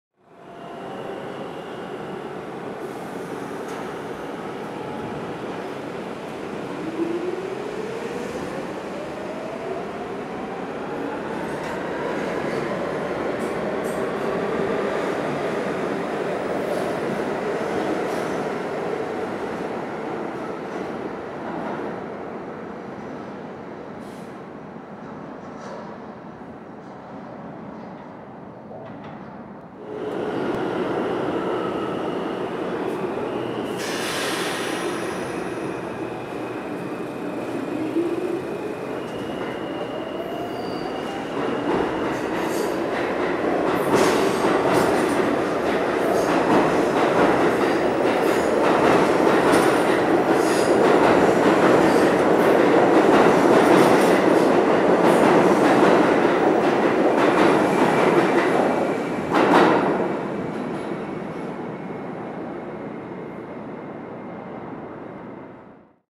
R46 F Trains Leaving Sutphin Boulevard
Some action on the F line at Sutphin Boulevard.

Taken on January 19th, 2014.